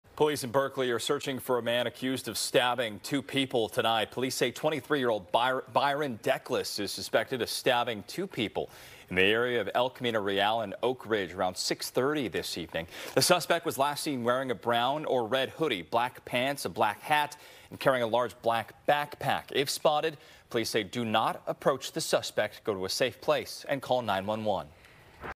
Shelter in place lifted as Berkeley police look for double stabbing suspect
Berkeley police are looking for a man suspected of stabbing two people near El Camino Real and Oakridge Steps, at the end of Oak Ridge Road, Friday evening.